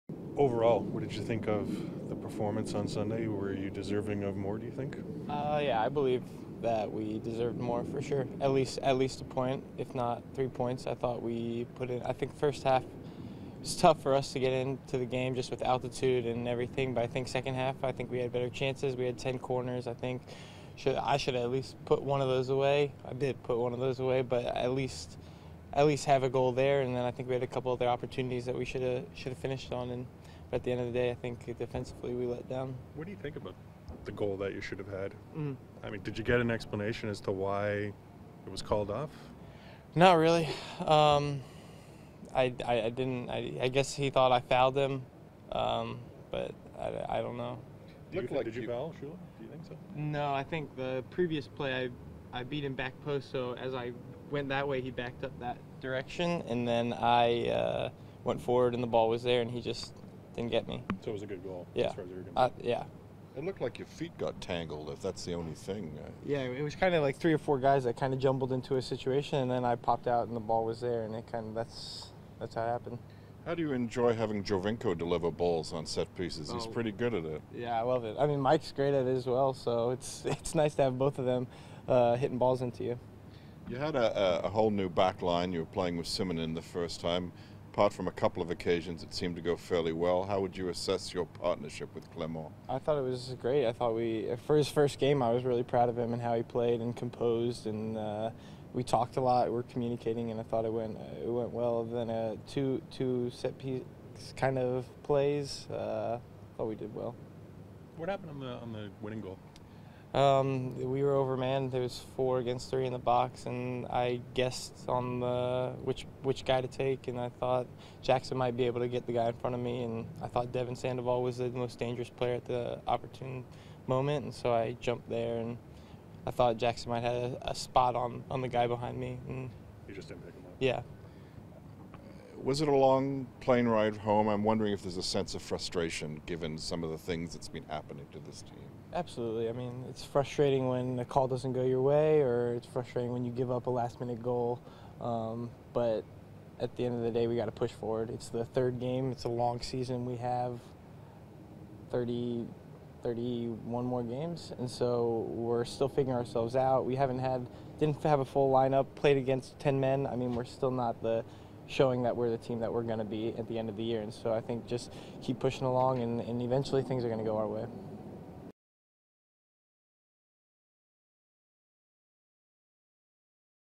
Nick Hagglund - March 31, 2015
Nick Hagglund speaks with members of the media following Tuesday's training session.